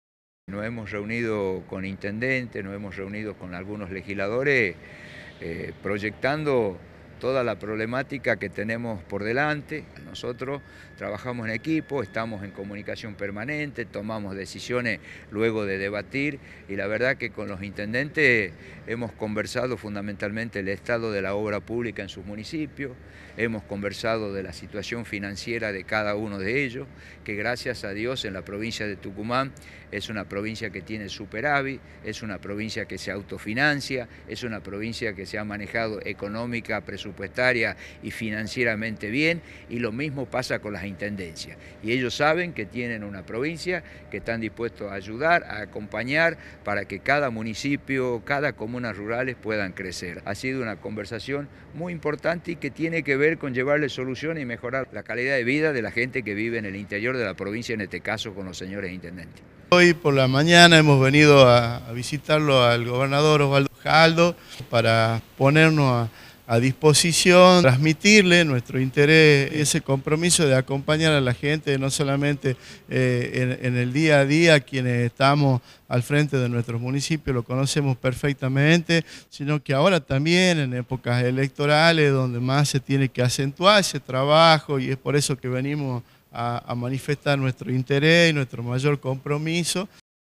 Osvaldo Jaldo | Reunión con intendentes de la jurisdicción oeste
𝗖𝗮𝘀𝗮 𝗱𝗲 𝗚𝗼𝗯𝗶𝗲𝗿𝗻𝗼 | Recibimos a los intendentes Javier Noguera (Tafí Viejo), Sandra Figueroa (Alberdi), Elia Fernández de Mansilla (Aguilares) y Leopoldo Rodríguez -h- (La Cocha) para dialogar acerca de los avances en cada una de las ciudades que conducen.

🤝🏻 A lo largo y ancho de la provincia estamos ejecutando obras y acciones gracias al compromiso de este gran equipo de trabajo que nos acompaña. Por eso apelamos al diálogo y el contacto permanente para llevar soluciones concretas.

🚧 Conversamos sobre el estado de la obra pública en cada municipio, sobre la situación financiera de cada una de las ciudades. En ese sentido, Tucumán es una provincia que tiene superávit, que se autofinancia y que se ha manejado económica, presupuestaria y financieramente bien; lo mismo pasa con las intendencias.

✅️ Esta es la línea de trabajo que queremos mantener y que vamos a sostener para que Tucumán siga creciendo.

👥️️ También estuvieron presentes el vicegobernador Sergio Mansilla,  el legislador Leopoldo Rodríguez y el secretario de Gobierno y Relaciones Institucionales, Luis Campos.